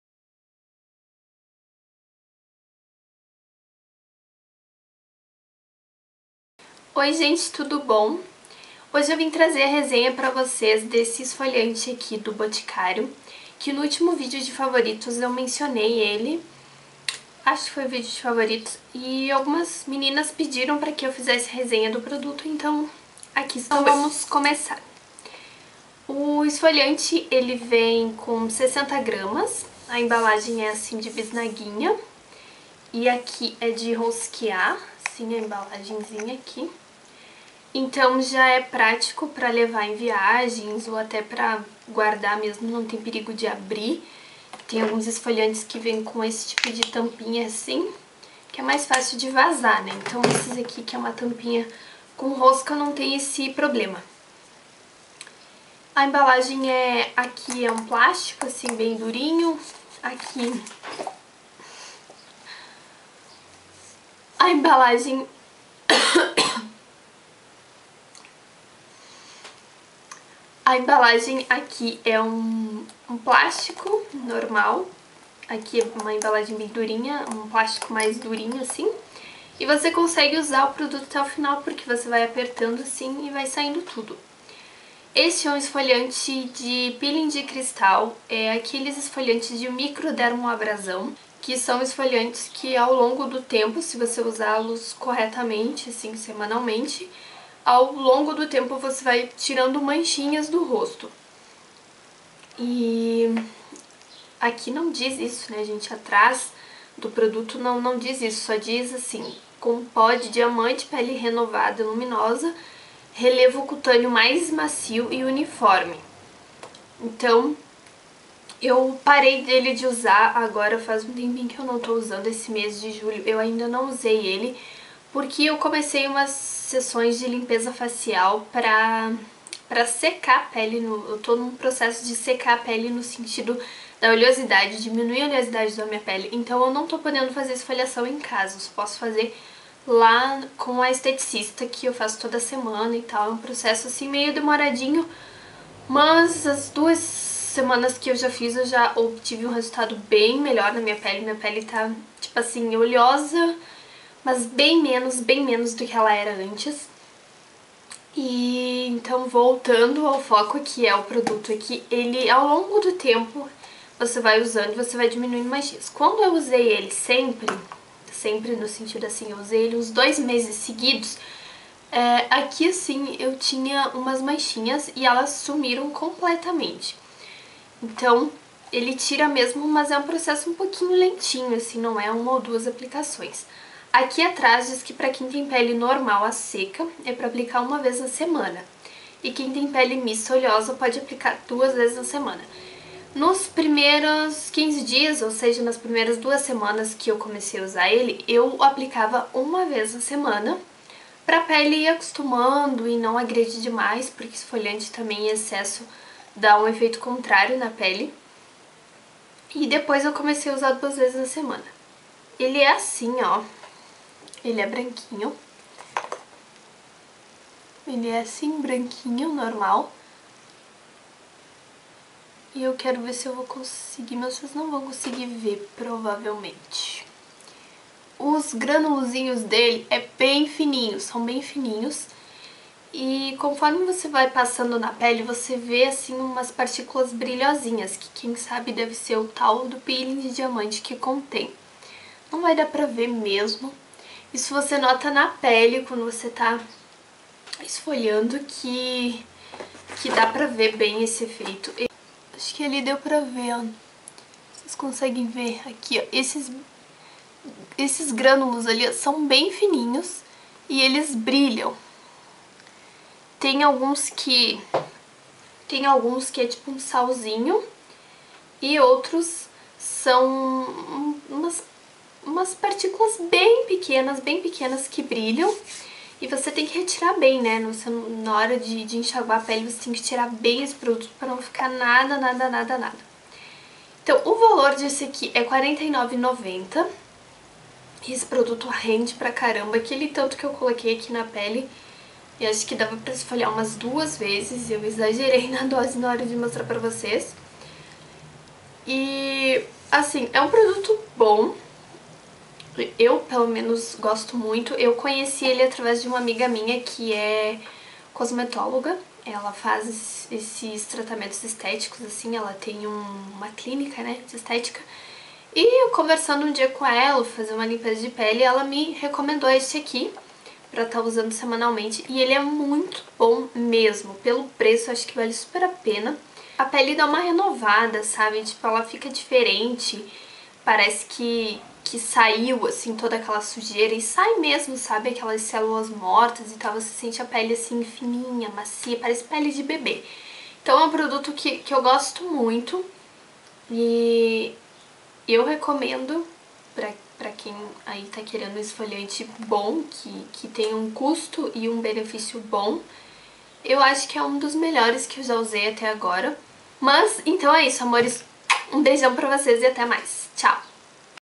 Esfoliante de Microdermoabrasão
Oi gente, hoje o vídeo é sobre um Esfoliante de Microdermoabrasão da linha Active Dermato, do O Boticário, algumas meninas pediram para que eu fizesse a resenha dele e aqui está! :) Beijooos